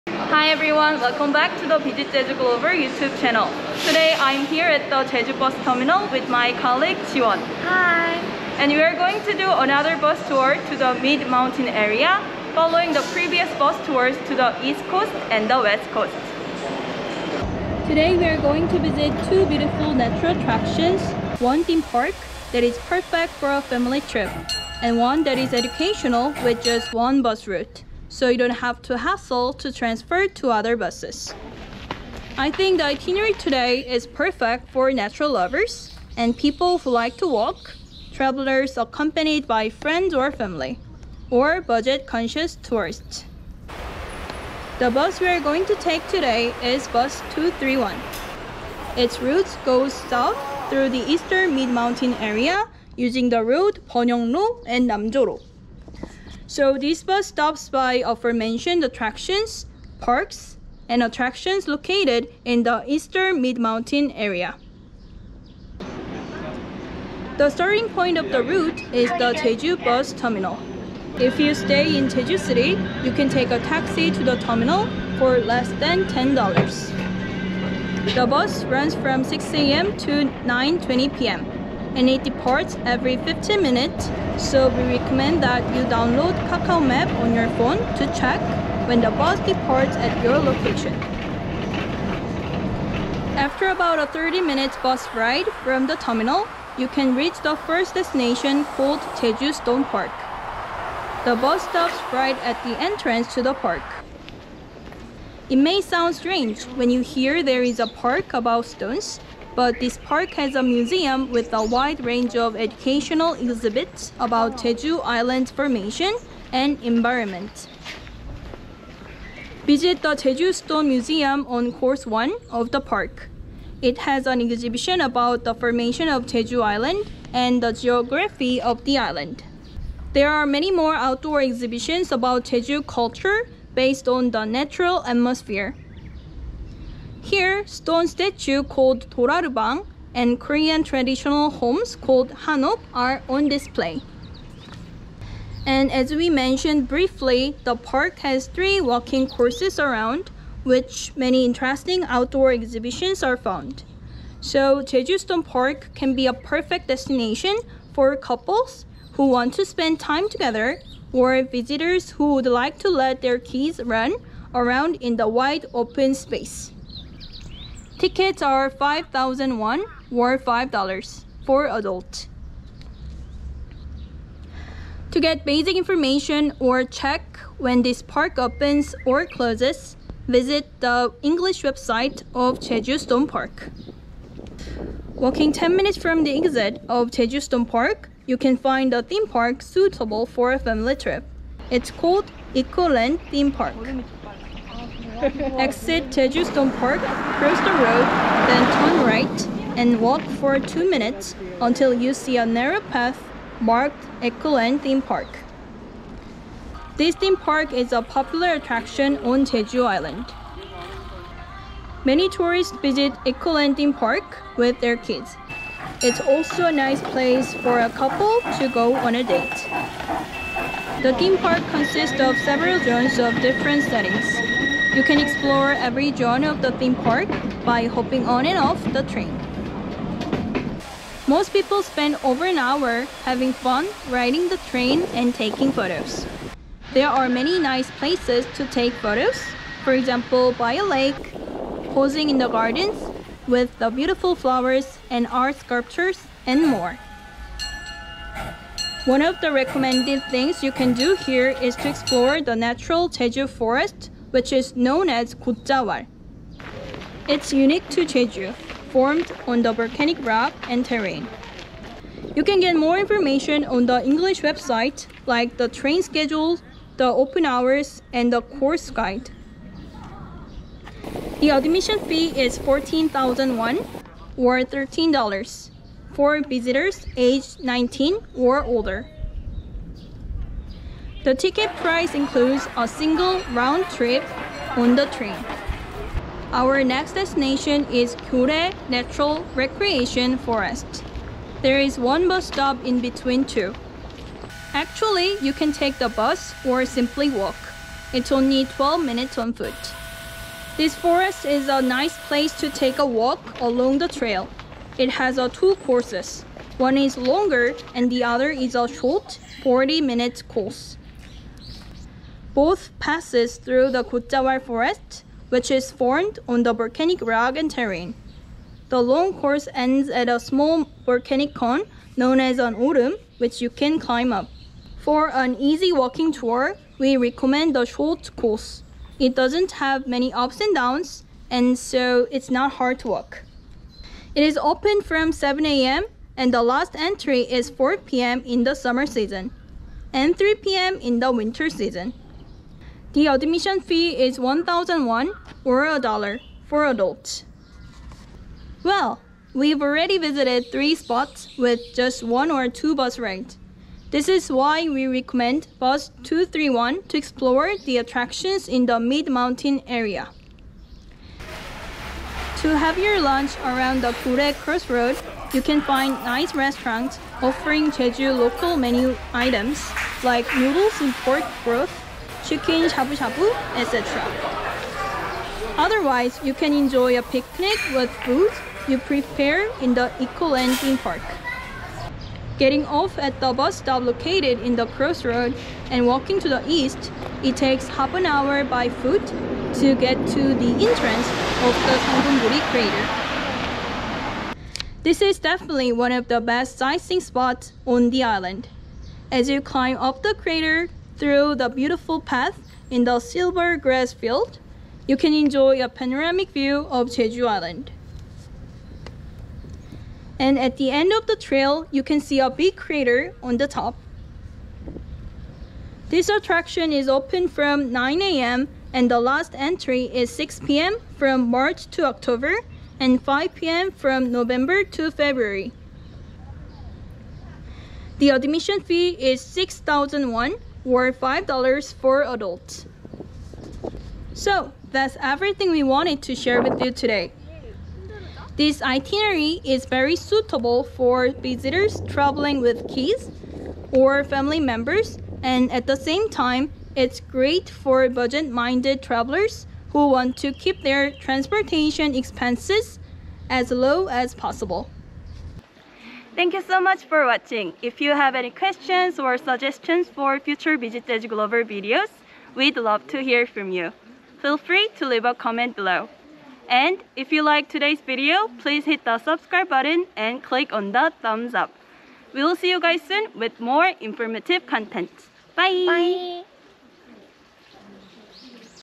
Jeju Easy Bus Tour: The Mid-Mountain Area
Here are the chapters,

00:00 Intro
01:33 Jeju Stone Park
03:05 Eco Land Theme Park
04:46 Gyorae Natural Recreation Forest
06:01 Lunch Ideas (Spicy fish stew, noodles in pork broth, chicken shabu shabu, chilled buckwheat noodles, etc.)
06:21 Sangumburi Crater

Hi, everyone. Today we’re back with another information-packed bus tour video. We traveled to the mid-mountain area of Jeju Island, visiting some family-friendly and natural destinations using just one bus: #231.

Bus #231 starts at Jeju Bus Terminal and runs from 6 a.m. to 9:20 p.m. We recommend that you download KakaoMap on your phone to check when the bus departs at your location.

We hope you will enjoy this video and find useful information for your next trip to Jeju Island.